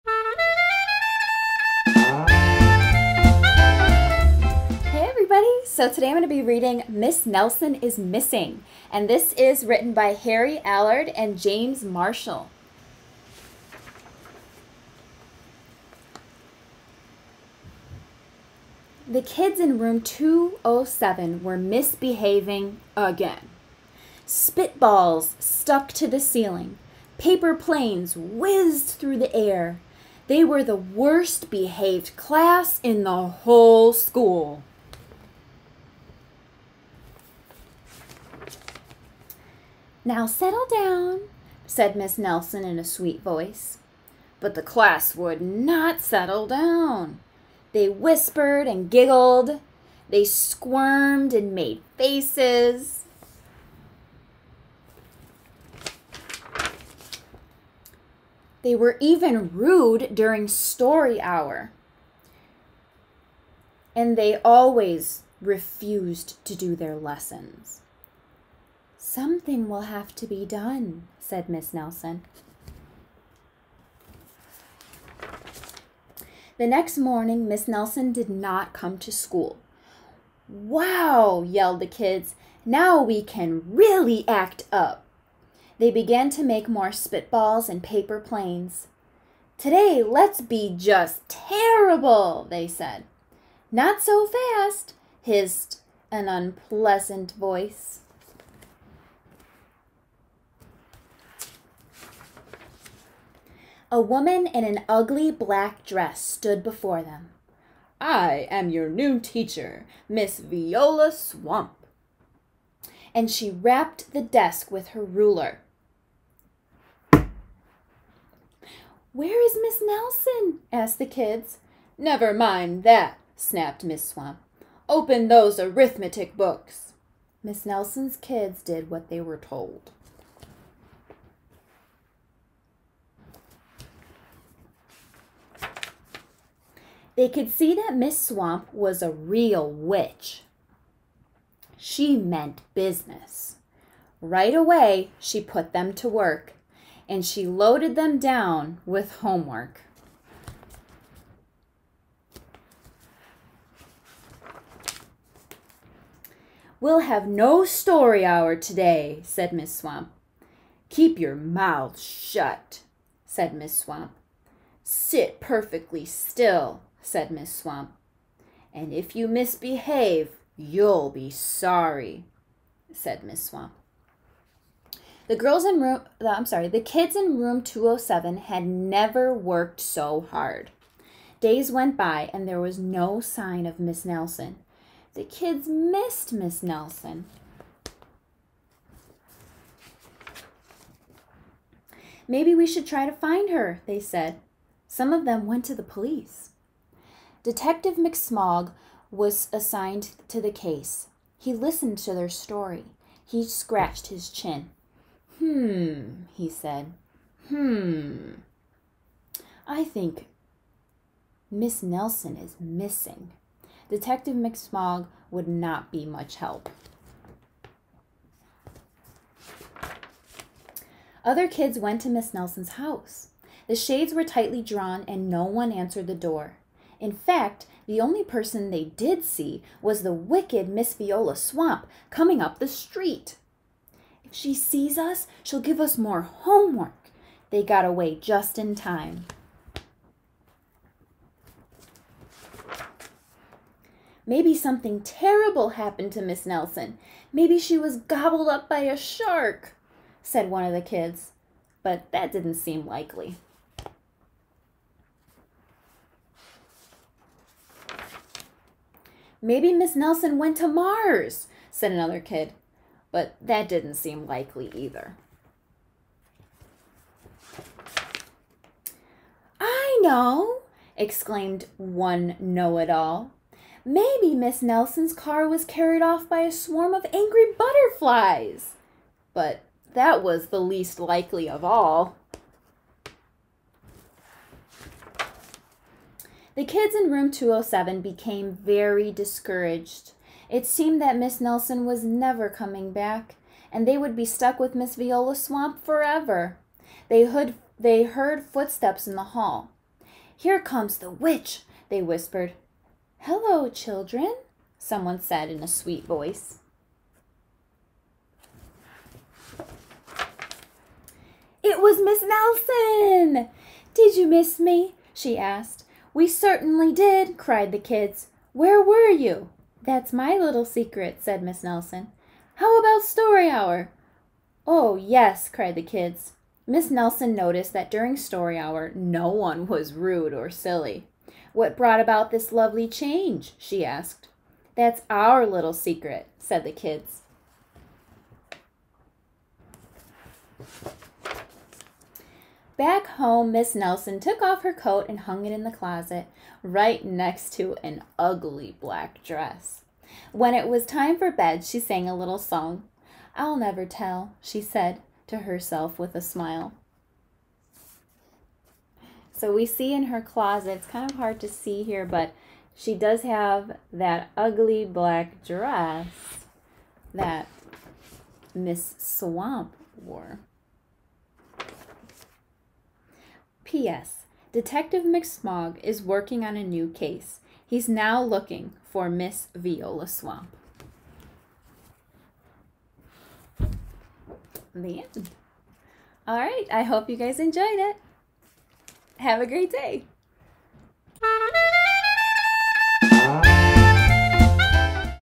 Ms. Bartucci's Read Aloud #15 ~ "Miss Nelson is Missing!" by Harry Allard
"Miss Nelson is Missing!" By Harry Allard